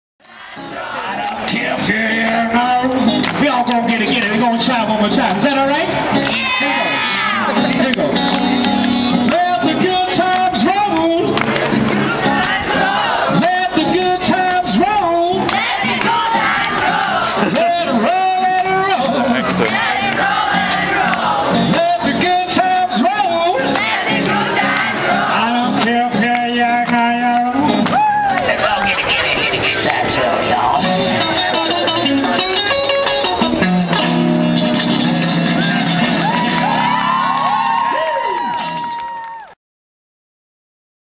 Singing with a street performer at Covent Garden!
The children from the boarding school experience visit Covent Garden and sing along!